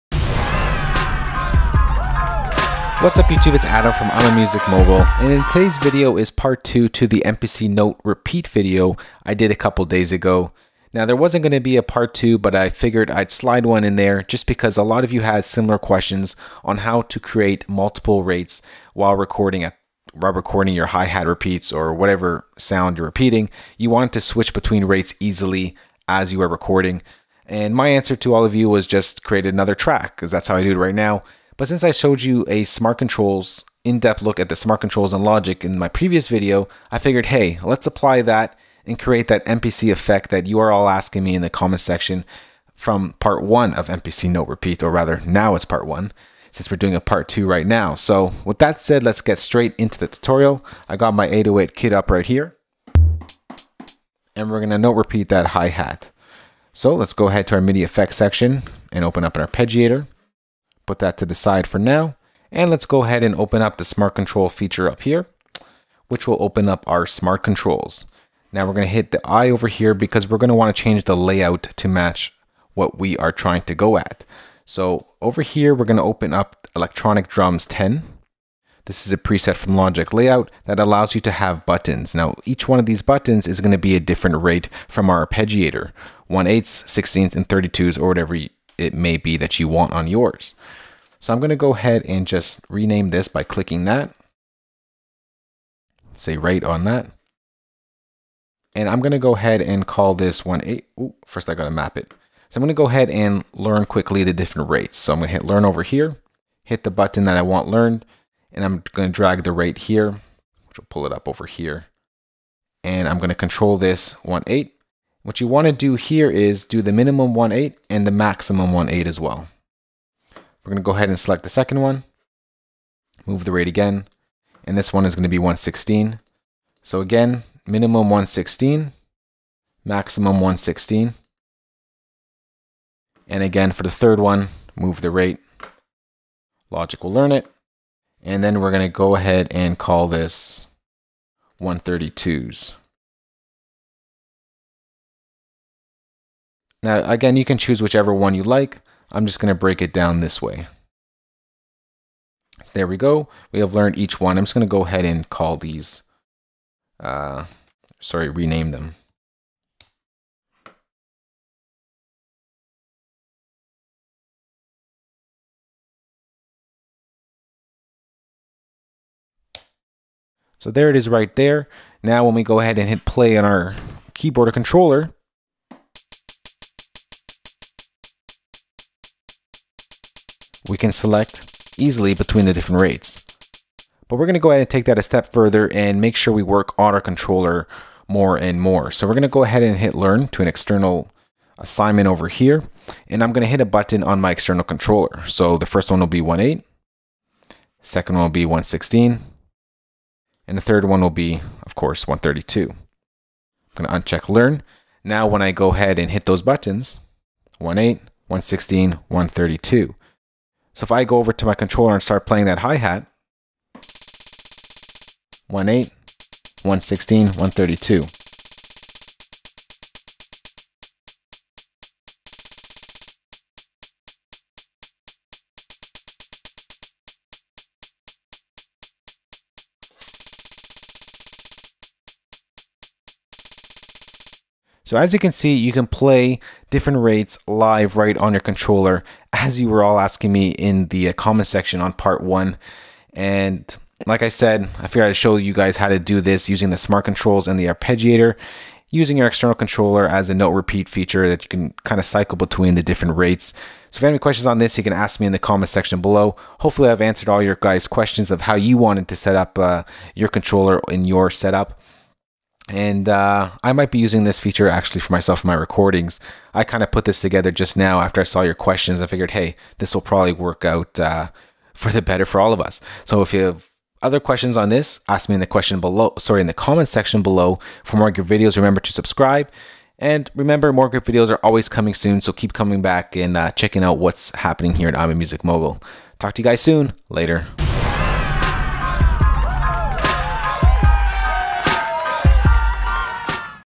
MPC Note Repeat in Logic Pro - Part 2 of 2
A few post ago, I covered how to achieve a MPC note repeat in Logic Pro. Many of you asked a way to control the different rates on your external controller while recording live. I took some time to work out an easy method for you all to get this in. I used to just create multiple tracks for the different rates and went on with recording.

The method we use in this tutorials surpasses just creating extra tracks and allows you to switch rates on the fly seamlessly. In this video we will be using Logics' built in Arpeggiator MIDI FX plugin combined with Logics Smart Controls to create a MPC note repeat.